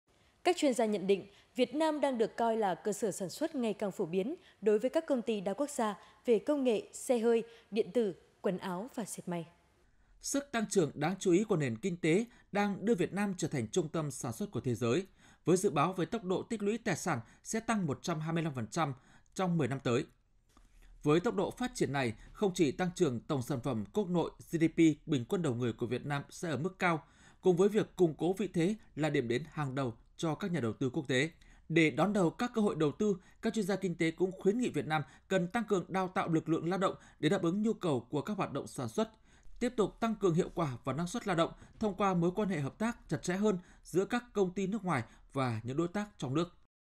Việt Nam là điểm đến ưa thích của các công ty đa quốc gia
Các chuyên gia nhận định, Việt Nam đang được coi là cơ sở sản xuất ngày càng phổ biến đối với các công ty đa quốc gia về công nghệ, xe hơi, điện tử, quần áo và dệt may.